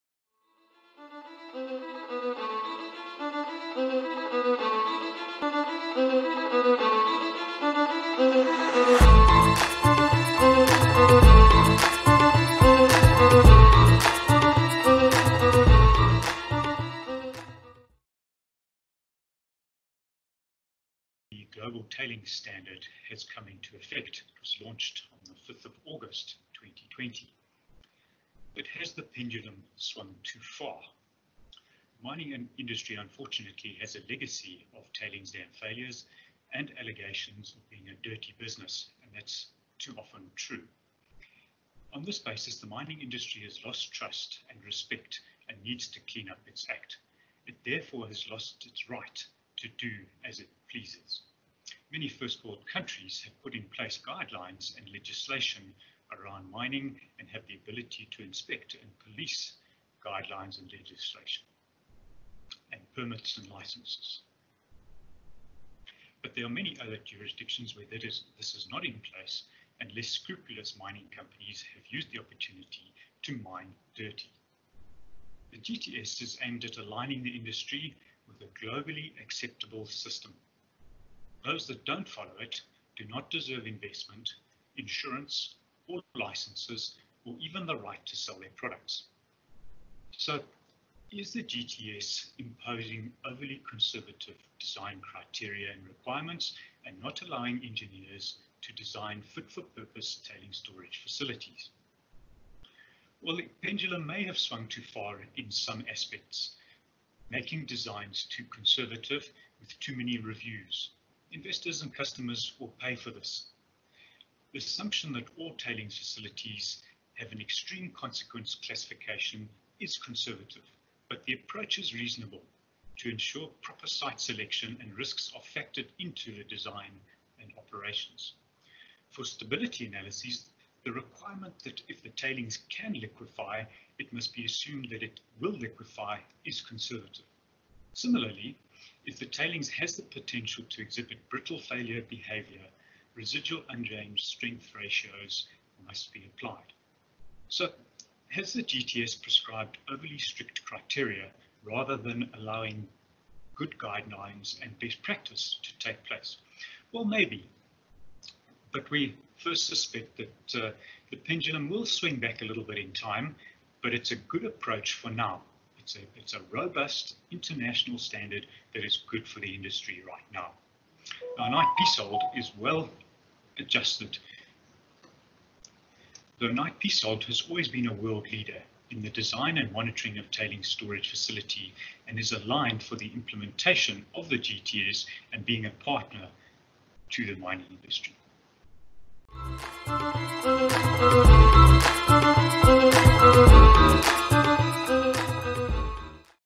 Part 1: Global Tailings Standard
Join the mining experts of Knight Piésold Southern Africa as they address the latest updates affecting the Mining Industry.

Today we have Andrew Copeland who looks at the new Global Tailings Standard launched on the 5 August 2020.

- Has the pendulum swung too far?
- Has the Global Tailings Standard (GTS) prescribed overly strict criteria and requirements rather than allowing good guidelines and best practice to take place?

Here’s what Tailings expert, Andrew Copeland has to say!